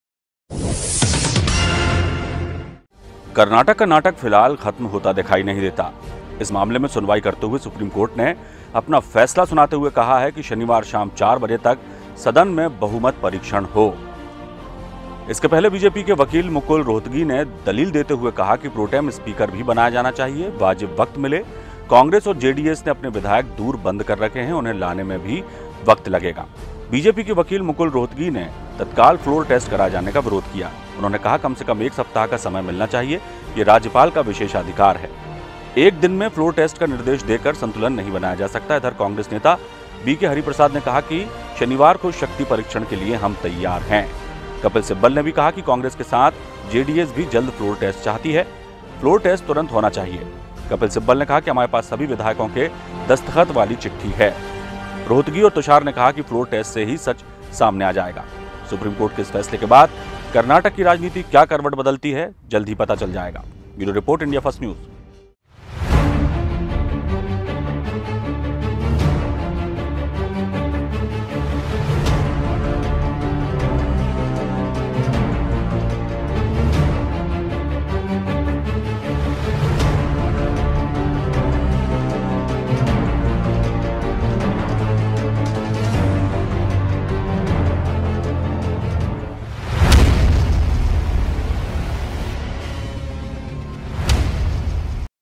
Karnataka High Voltage Politics . कर्नाटक का ताज़ा नाटक - देंखे 1 मिनिट में ।
कर्नाटक का नाटक फिलहाल खत्म होता नही दिखाई देता इस मामले में सुनवाई करते हुए सुप्रीम कोर्ट ने अपना फैसला सुनाते हुए कहा है कि शनिवार शाम 4 बजे तक सदन में बहुमत परीक्षण हो.